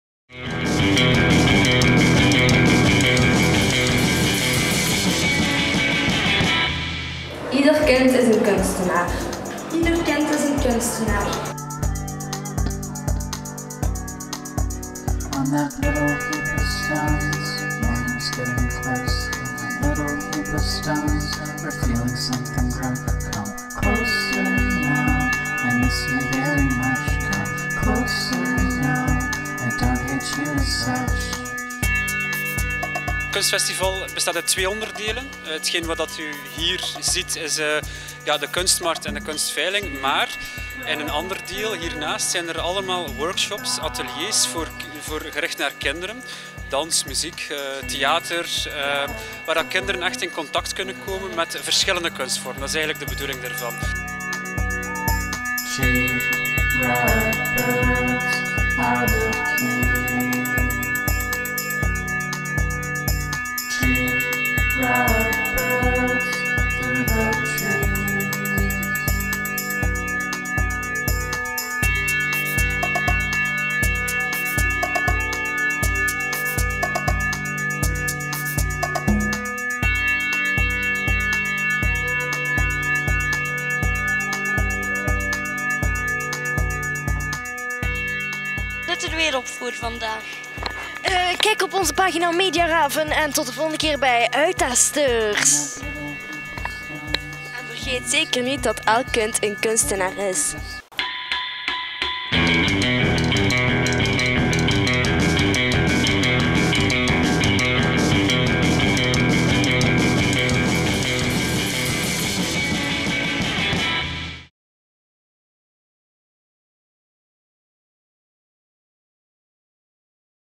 UITtesters proeven van het Uilenspel Kunstfestival
Het Uilenspel kunstfestival is een kinderfestival waar kinderen en jongeren kunnen proeven van verschillende kunstdisciplines en kunstzinnige activiteiten. Op zaterdag 30 november 2013 vond de allereerste editie plaats. Daar konden de UITtesters natuurlijk niet op ontbreken.